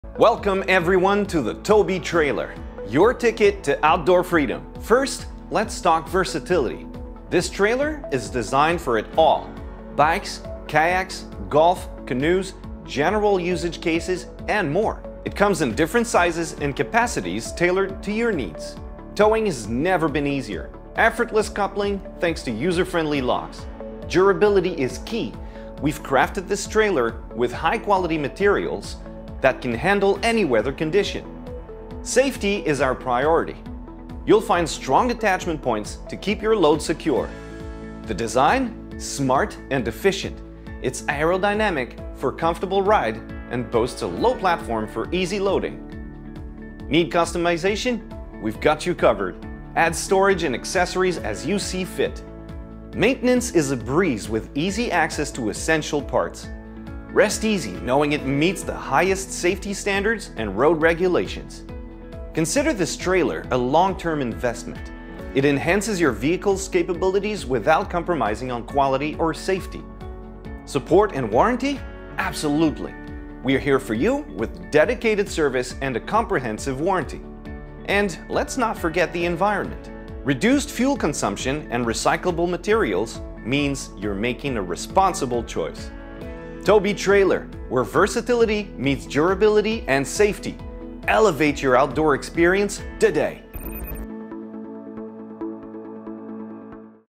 Tow-Bii Trailer Presentation by Velocity Distribution
TOW-BII, established in 2015, is driven by a mission to deliver durable, reliable, and customized trailers that cater to the unique requirements of its customers. The TOW-BII multi-functional motorcycle & auto trailer, manufactured and assembled exclusively in Quebec, reflects the company's unwavering commitment to exceptional craftsmanship and harnessing local expertise.